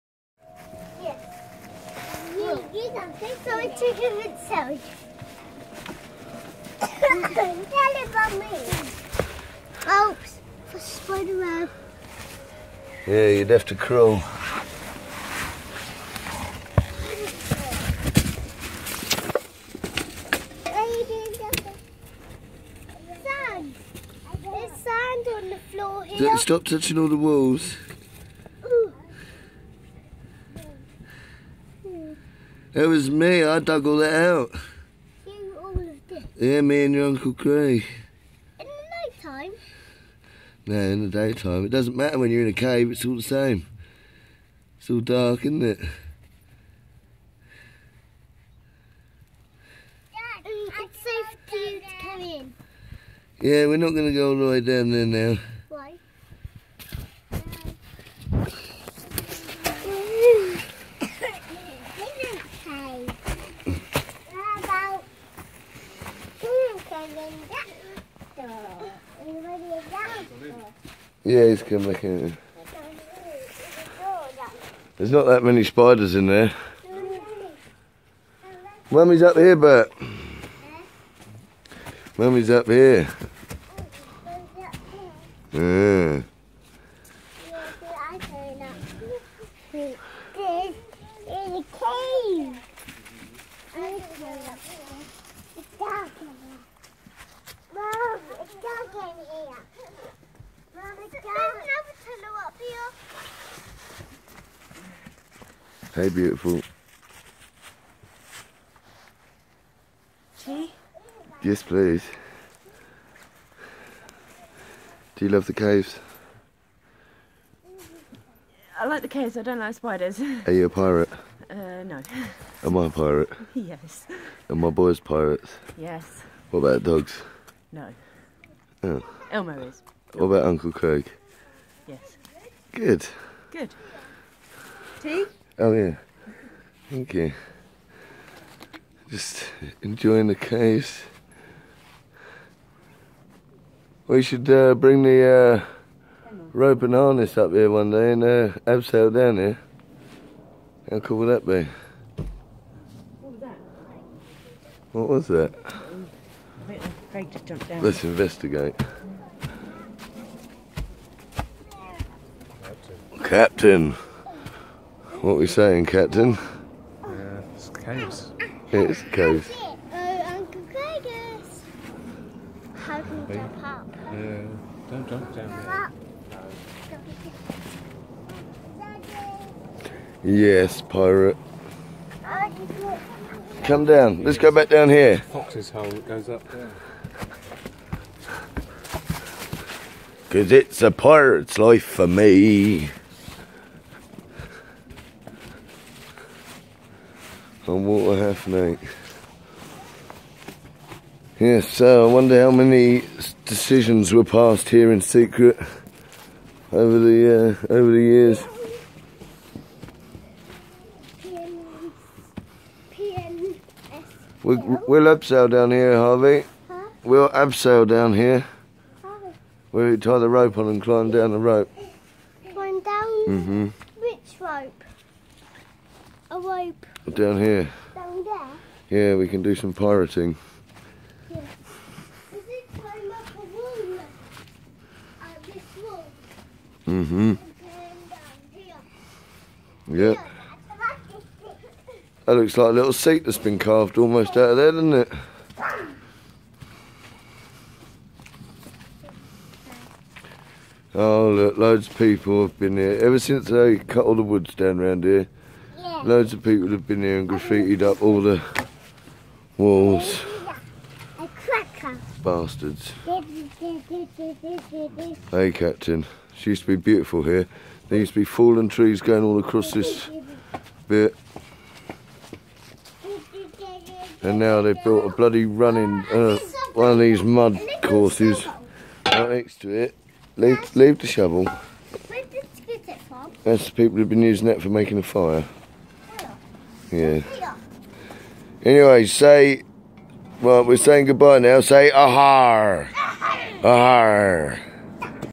SECRET CAVES WALK AROUND.
Pirates explore around illuminati caves